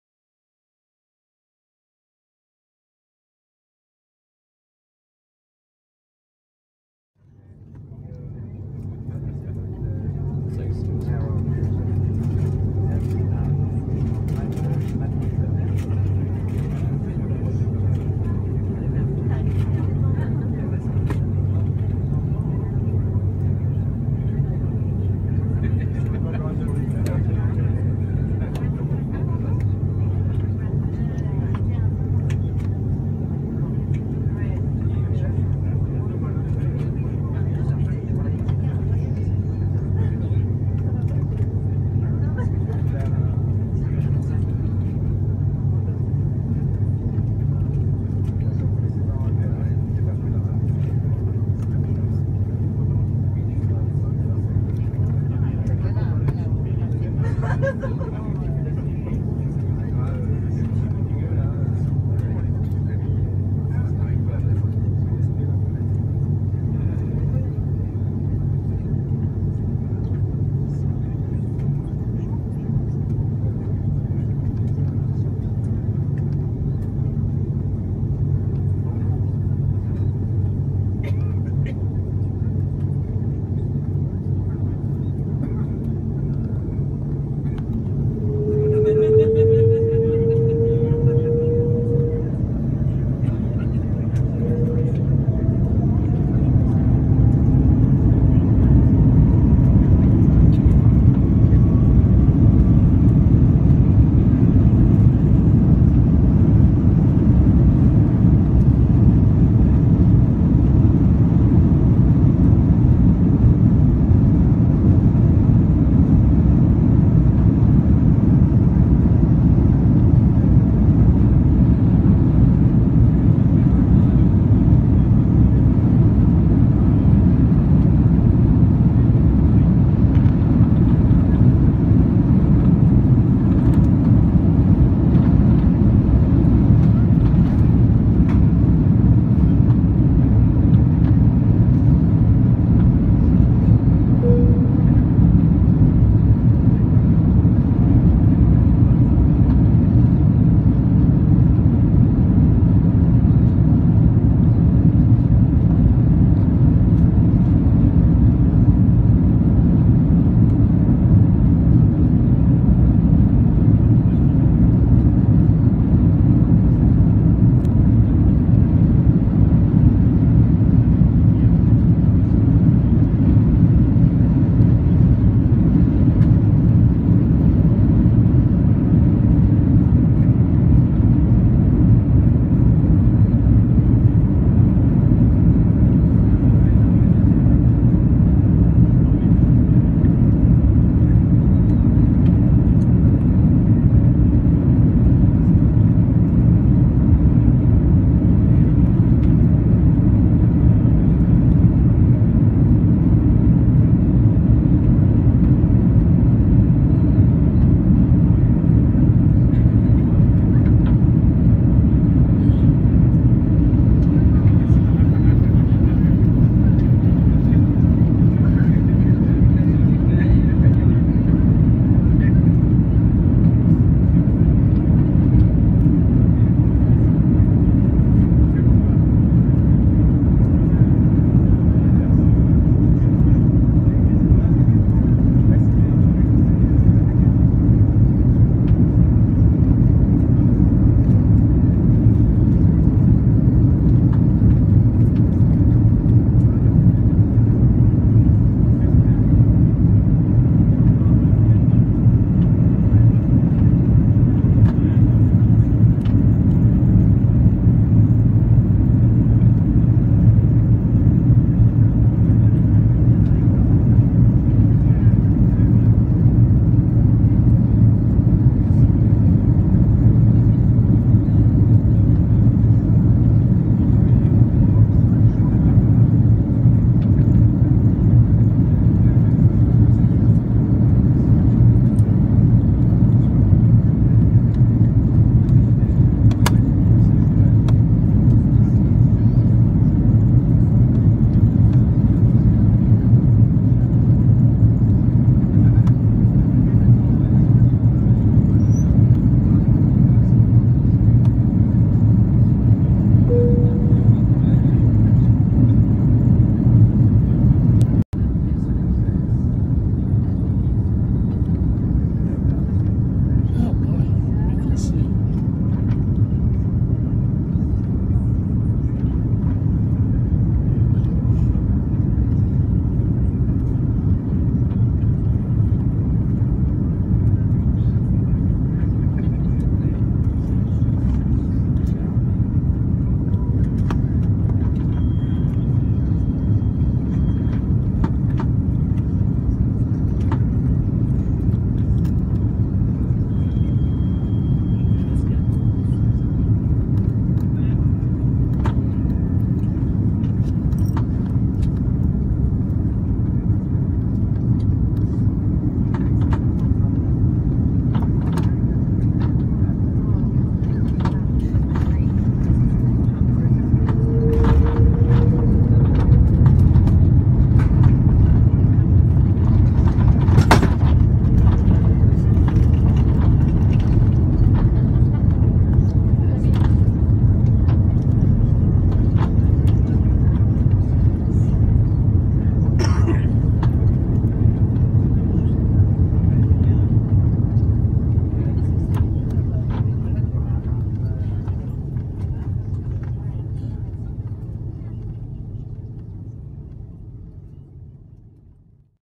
ACA419 YUL - YYZ C-GHKW 01/10/2016
Onboard Air Canada A330 C-GHKW from Montreal to Toronto.